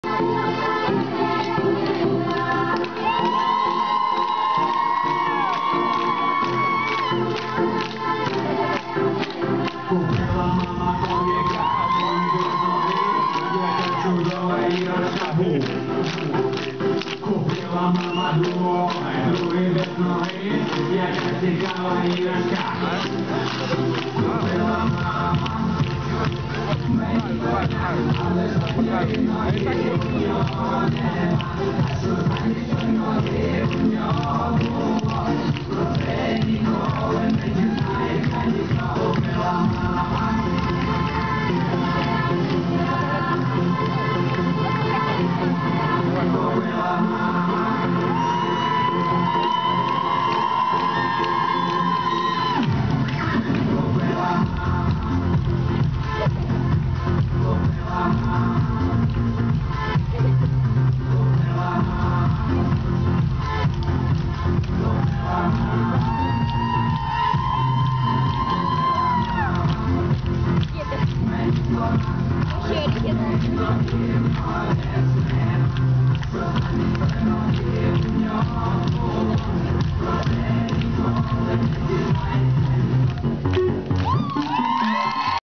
Майданс. г. Кировское СОШ №1. Танец хореографов.
танец хореографов команды " Танкисты"
На видео Владислав Хмиль, Ринат Ризванов, Алёна Галяутдинова.